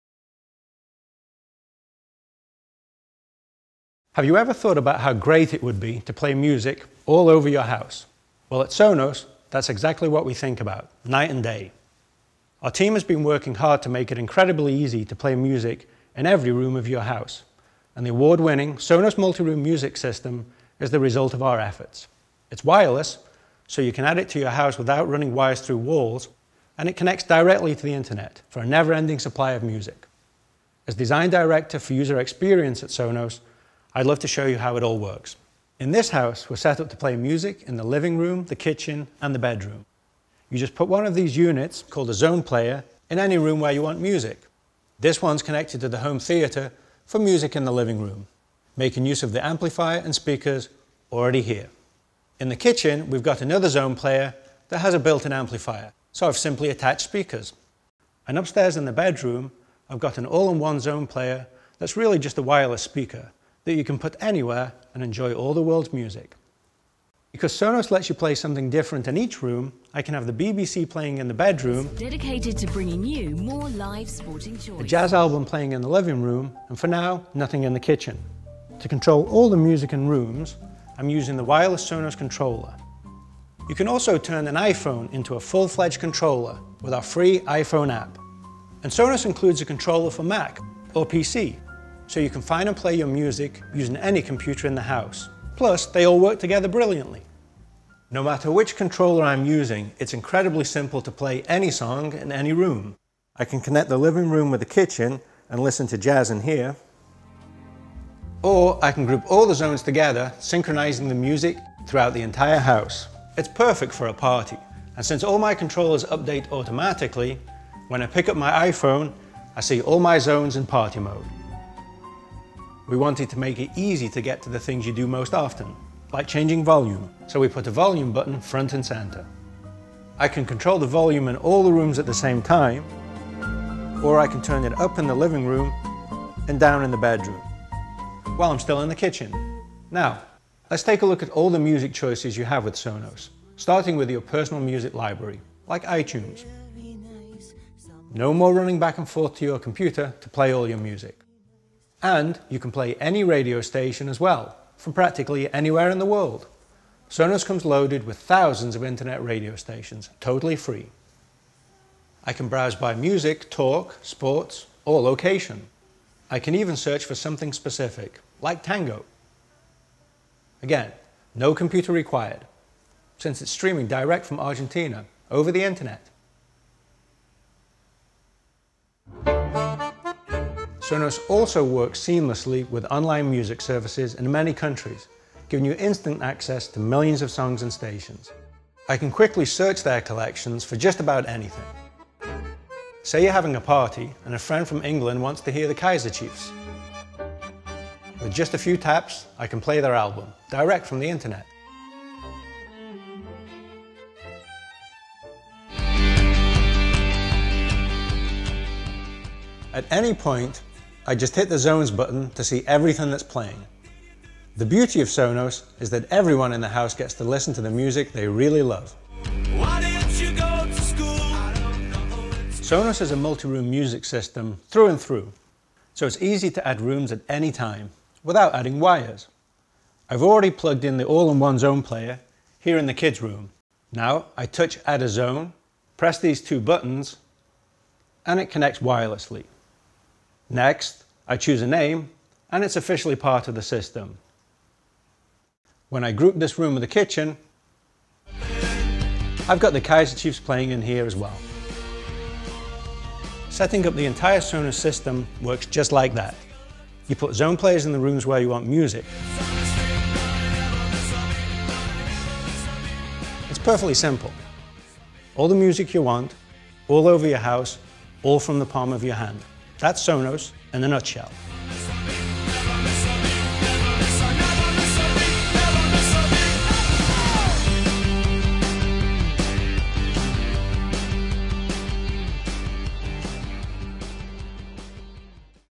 Sonos System - how it work!
The Sonos Controller 200 gives you instant access to all your music and all your rooms at the touch of a finger. Just pick it up or touch the screen and youre in total control. Simply flick to browse, type to search and touch to play. You can turn off the upstairs music from downstairs or change the song in the kitchen from the den. And with a single touch you can play the same song in every room or different songs in different rooms. Best of all, every Sonos Controller 200 works wirelessly and responds instantly — no aiming or waiting required.